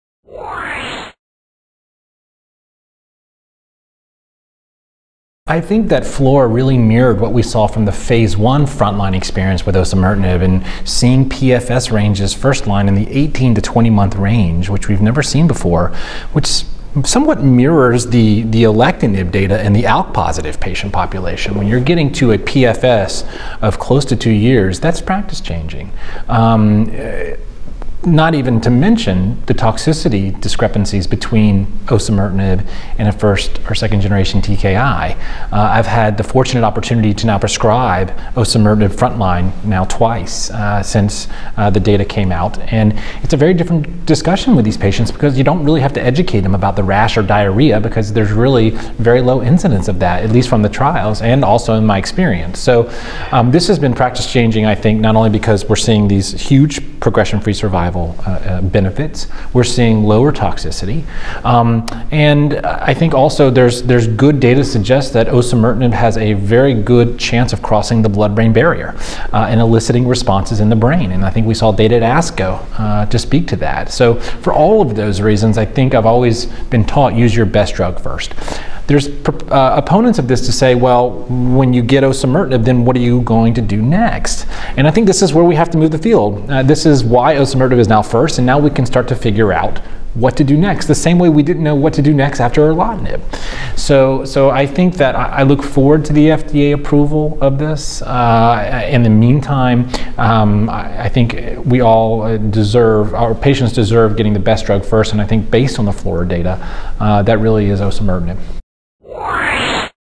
Dr. Levy Discusses the FLAURA Study in Lung Cancer
Benjamin P. Levy, MD, assistant professor of oncology, clinical director of medical oncology, Johns Hopkins Sidney Kimmel Cancer Center, Johns Hopkins Medicine, discusses the FLAURA study for patients with lung cancer.